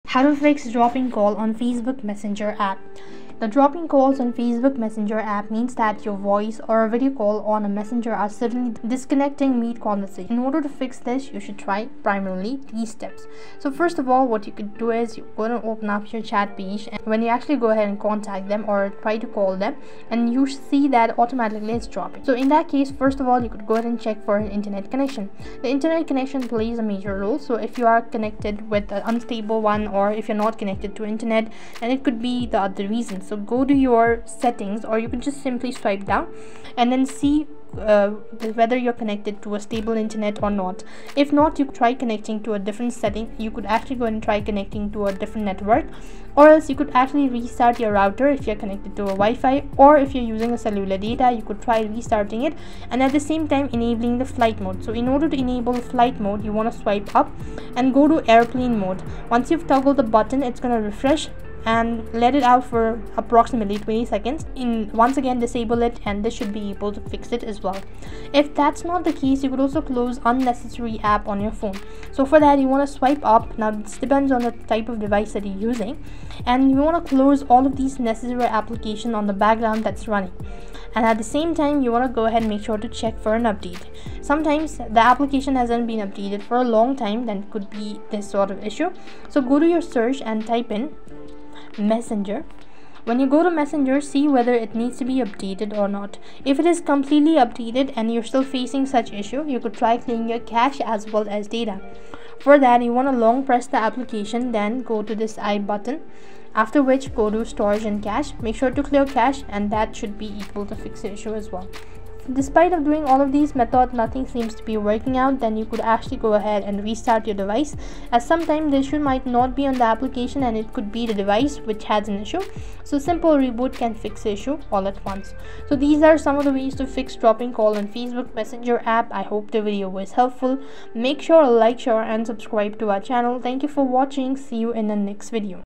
How To Fix Dropping Calls On Facebook Messenger App
If your calls keep dropping on the Facebook Messenger app, it could be due to an unstable internet connection, app glitches, outdated software, or background apps using too many resources. Here are some troubleshooting steps to fix the issue.

First, check your internet connection. A weak or unstable connection can cause calls to drop. Go to your device’s settings or swipe down on your screen to check your Wi-Fi or mobile data status. If you're on Wi-Fi, try restarting your router. If you're using cellular data, toggle airplane mode on for about 20 seconds and then turn it off to refresh your network connection.

Next, close unnecessary background apps. Too many apps running at once can slow down your device and affect call stability. Swipe up or use your device’s task manager to close any apps you're not using.

If the problem persists, check for app updates. An outdated Messenger app can cause connectivity issues. Open your App Store or Play Store, search for "Messenger," and tap "Update" if an update is available. After updating, restart the app and try calling again.

Another solution is to clear the app’s cache. Long-press the Messenger icon, select "App Info," go to "Storage & Cache," and tap "Clear Cache." This removes temporary files that might be causing the issue.

If none of these solutions work, restart your device. Sometimes, the problem isn't with the app but with your phone’s system. A quick reboot can resolve many performance issues.

By following these steps, you should be able to fix call-dropping issues on Facebook Messenger. If the problem continues, consider reinstalling the app or contacting Messenger support.

Timestamps:
0:00 Introduction
0:14 Checking internet connection
0:40 Restarting router or enabling airplane mode
1:06 Closing background apps
1:22 Updating the Messenger app
1:39 Clearing app cache
1:55 Restarting your device
2:07 Conclusion